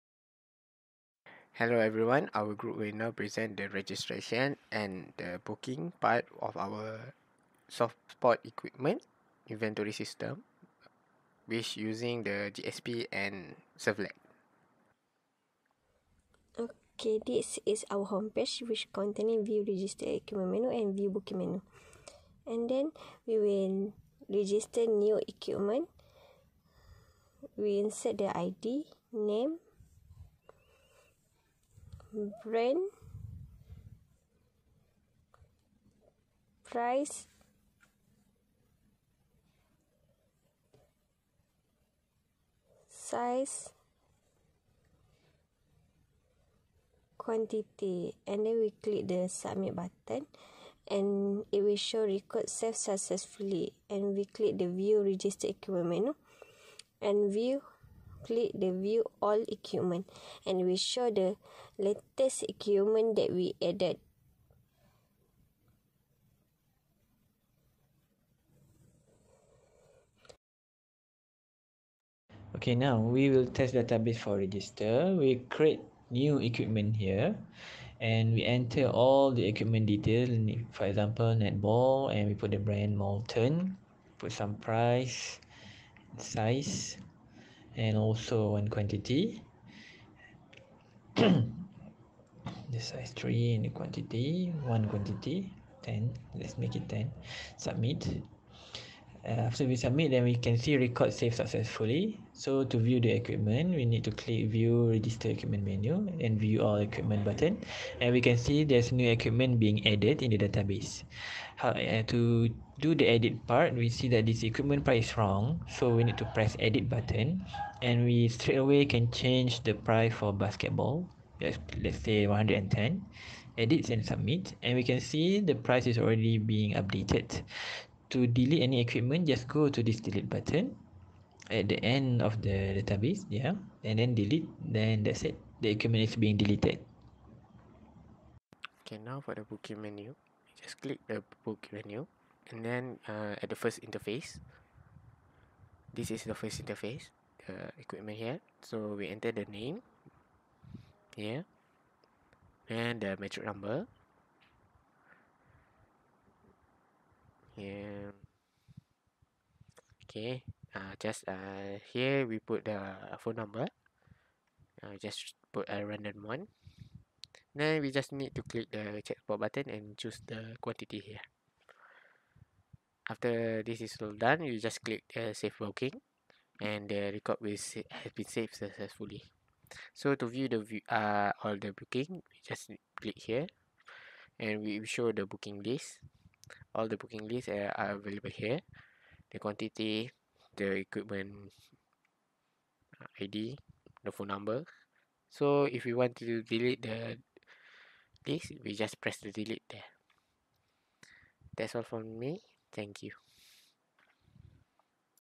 Project 2 Assignment - Sport Equipment Registration and Booking System
BITP3123 DISTRIBUTED APPLICATION DEVELOPMENT

Development of application using JSP and servlet approach.

Summary: The system handles CRUD for the equipment registration and booking system using JSP and Servlet approach